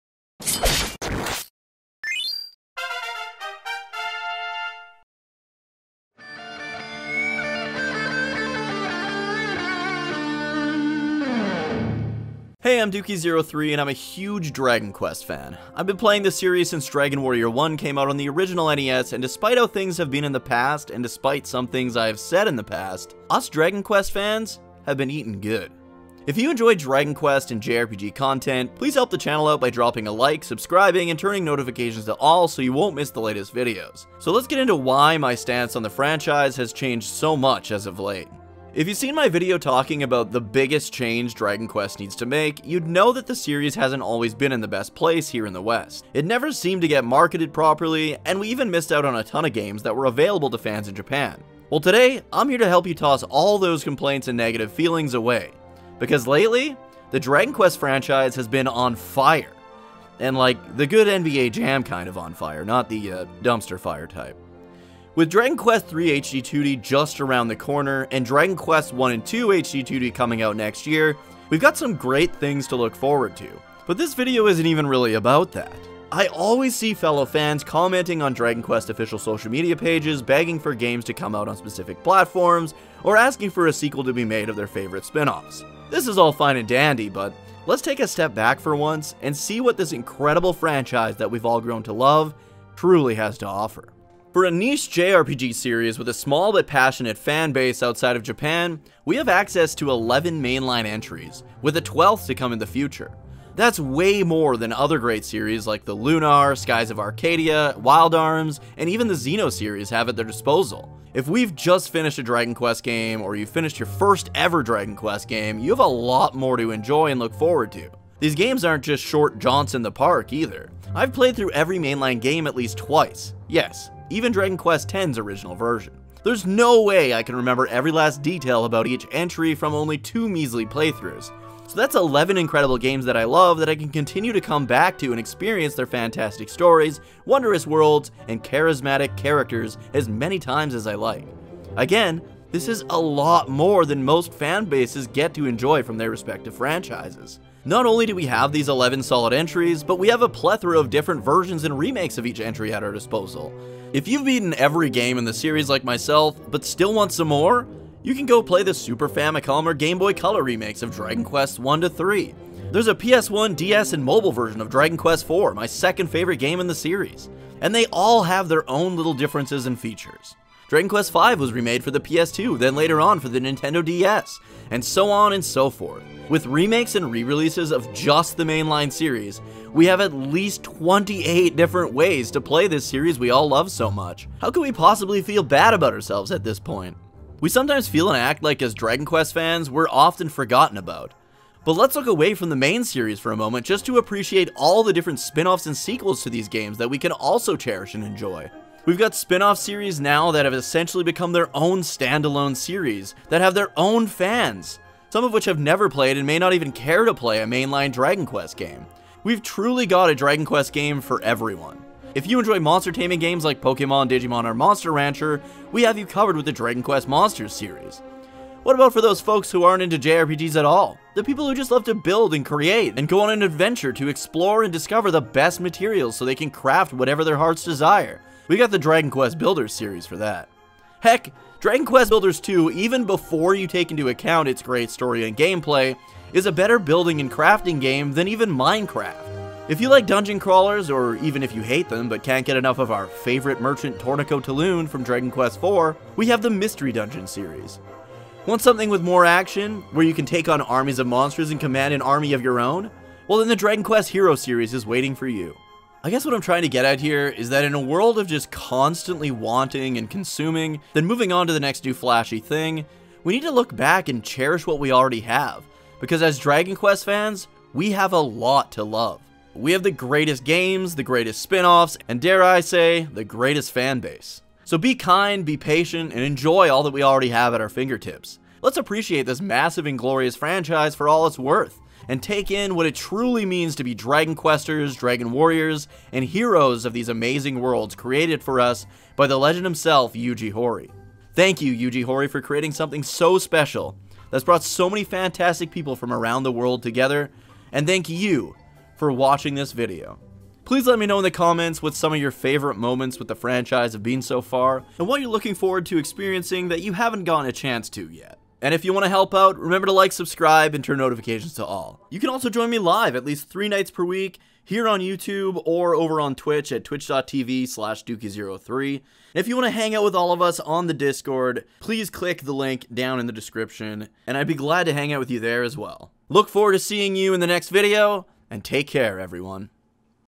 Dragon Quest Fans Are EATING GOOD!!
This is the BEST time to be a fan, or get into the series for the first time!

Timestamps:
0:00 - Intro
0:38 - Why We Felt Betrayed
0:55 - Great Things To Come!
1:26 - What Dragon Quest REALLY Has To Offer!
2:27 - SO MUCH VARIETY!
3:07 - INCREDIBLE Spin Offs!
4:15 - We Have SO MUCH To Love!
5:00 - Favourite Moments?